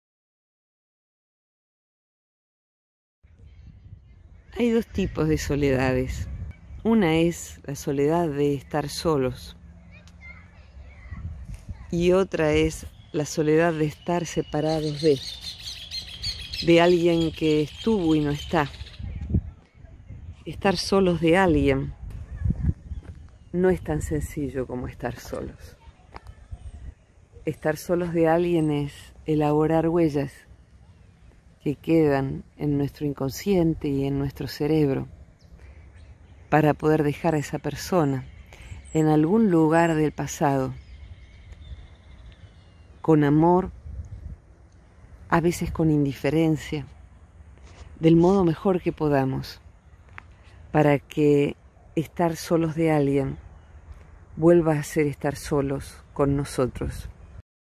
Virginia Gawel: DOS TIPOS DE SOLEDADES
LA SOLEDAD QUE ES SOLITUD es una generación propia: se la gesta, se la pare, se la cuida como a un niño, para que se afiance y madure.

Es una soledad que no se agarra de nada ni de nadie: habita el vacío hasta no temerle, sino volverlo útero de nuevos parimientos.

No saber estar en solitud es como tener el sistema inmunológico en déficit: nos volvemos propensos a que la compañía equivocada nos intoxique (e inclusive que no lo advirtamos!).

Y si lo que se ha perdido nos deja solos sin solitud, solos en soledad (como cuando muere o se va alguien muy amado), el trabajo será darse tiempo y procesar, zurciendo la cicatriz, zurciendo con lúcida sensibilidad...

Pero la mayoría de nuestras cicatrices suelen no ser por alguien que valiera tanto zurcimiento: debían ser no más que para el cielo ese tajo de nube.

Mucho por lo que lloraste y por lo que lloré solo fueron tajos de nube para nuestro cielo. Estate contigo: soledad en plenitud da solitud (y compañía elegida... no "agarrada"!!). . ¿Tiene este tema algo que ver con tu vida? Te escucho... y te abrazo!

Virginia Gawel:
Centro Transpersonal de Buenos Aires